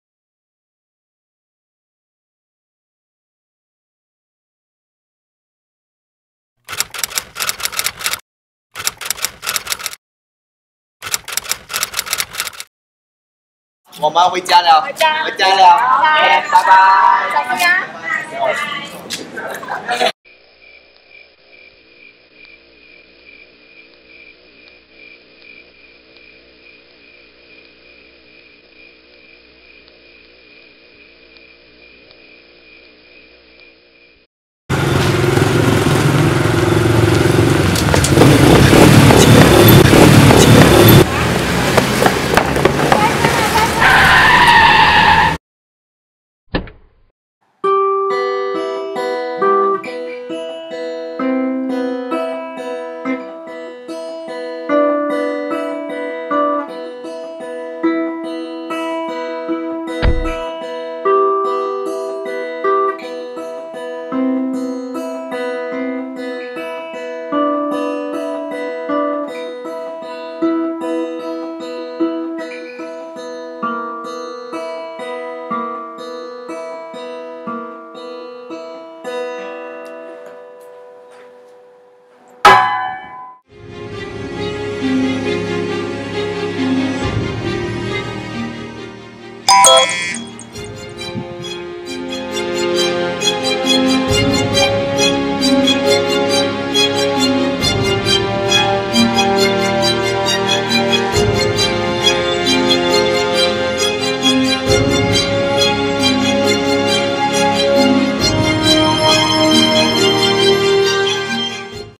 DISTED M.E.P. Trailer
"Your leader is caught and the only way for you to save him or her is by working together to complete a series of missions. Will you be able to save your leader and run for your lives?"

Mission E-Possible or also known as, Mission Escape Possible is a theme based station games.